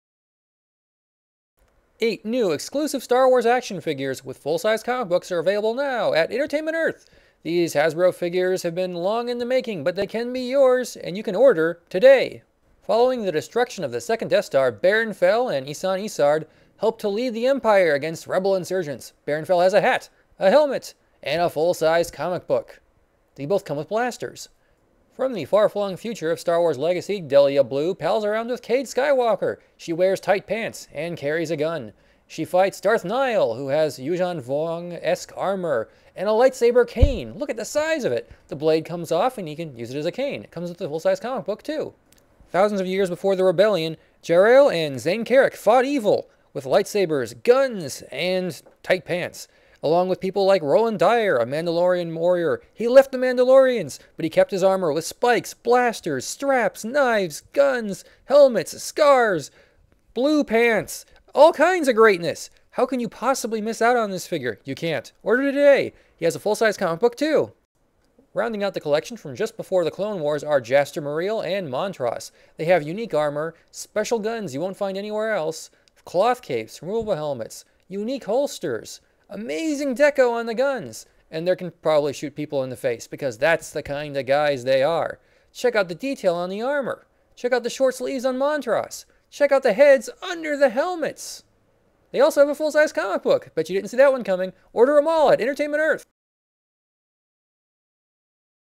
Star Wars Action Figure Comic Packs - An Entertainment Earth Exclusive
- these 8 action figures comic with full-size comic books and great accessories.  Enjoy Mandalorians, Sith, Imperials, and more!